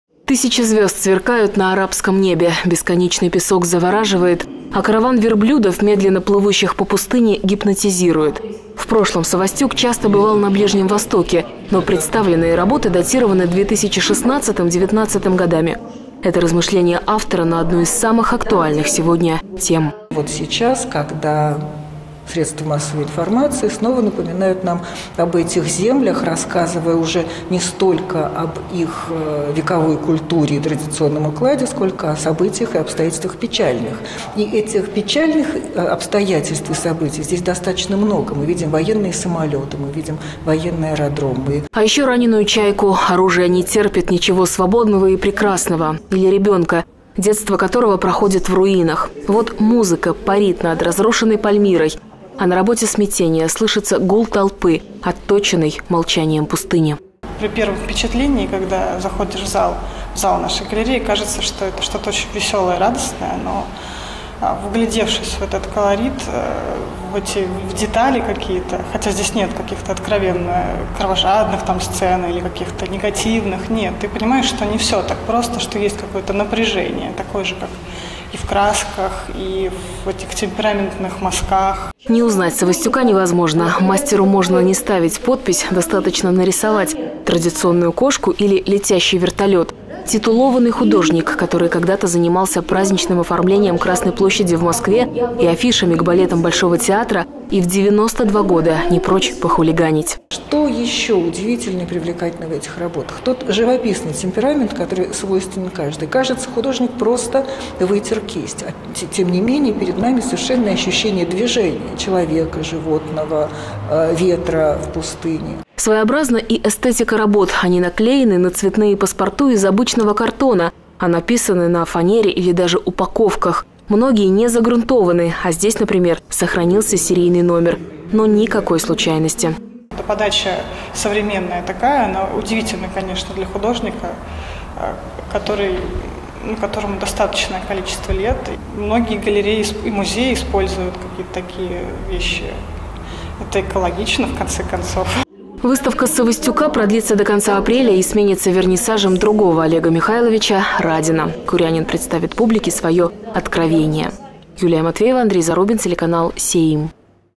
"Караван". Народный художник Олег Савостюк представил в Курске выставку о Ближнем Востоке.
В Курской картинной галерее работает выставка живописи народного художника РСФСР, академика, выпускника института имени Сурикова Олега СавостюкА. Это уже третий вернисаж мастера в нашем городе, в этот раз он посвящён теме Ближнего Востока. «Караван» - это проиллюстрированный рассказ о древней истории, высочайшей культуре, о традициях и, в то же время, переменчивом укладе жизни. Сюжет Юлии Матвеевой.